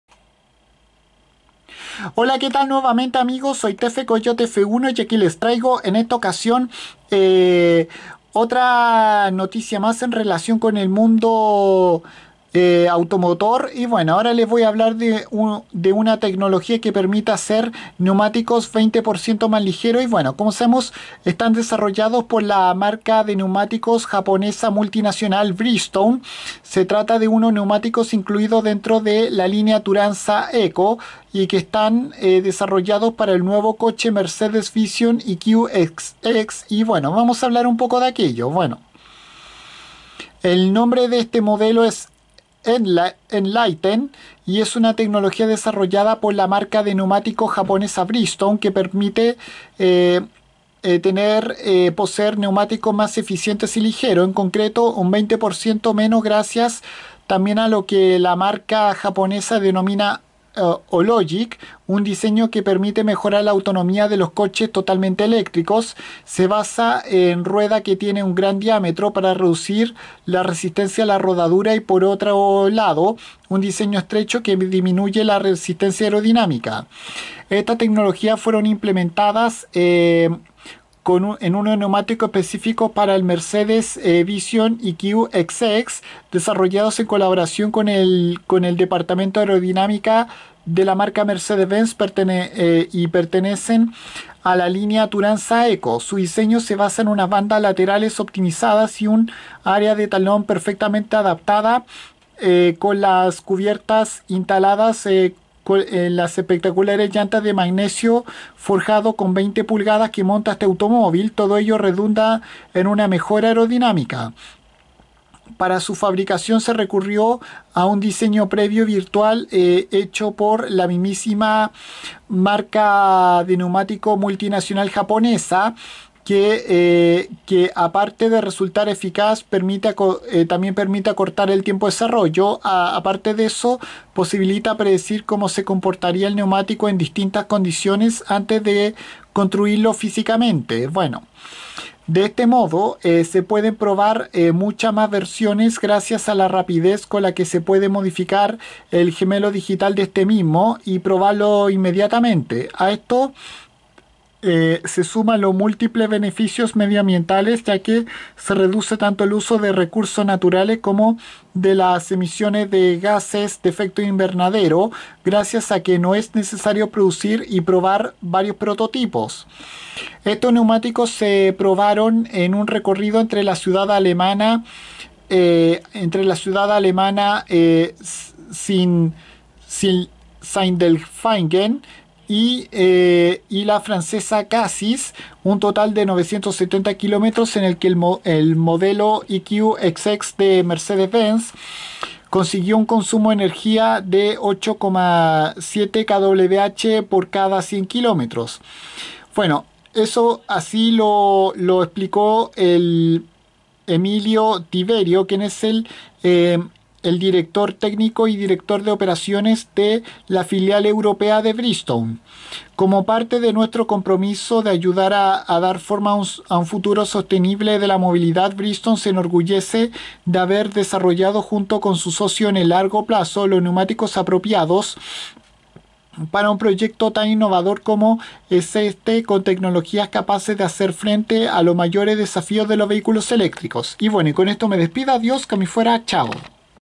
Bridgestone presenta unos nuevos neumáticos que 20% mas ligeros
La marca de neumáticos multinacional japonesa los desarrolló para su línea Turanza ECO y se desarrollaron exclusivamente para el futuro coche deportivo totalmente eléctrico Mercedes Vision EQXX